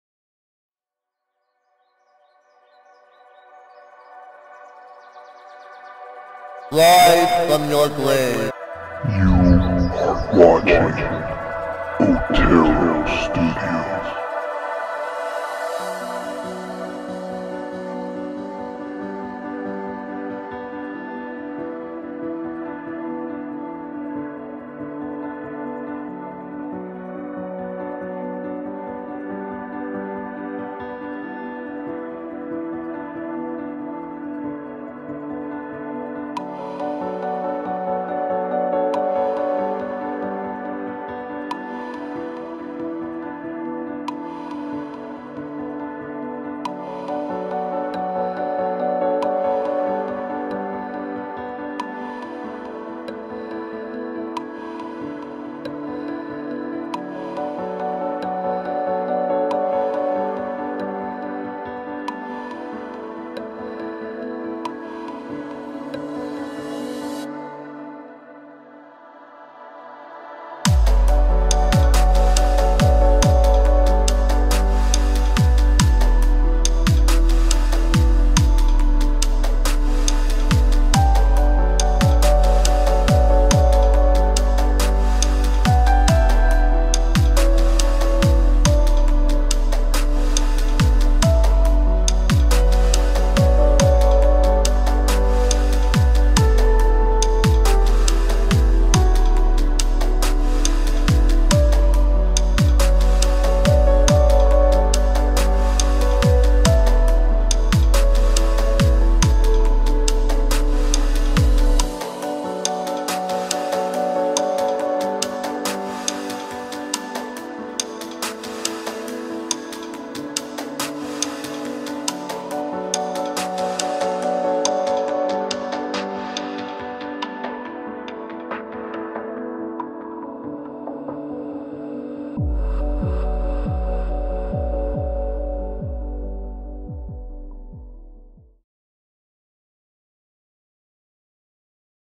Procreate Timelapse Videos in one Tomasso Ciampa , Bianca Belair and Finn Balor
some videos from my ipad procreate app

Otero studios
ArtStation @oterostudios

Tools:
IPad Pro
Canon m50
Sony cx 445
Copic markers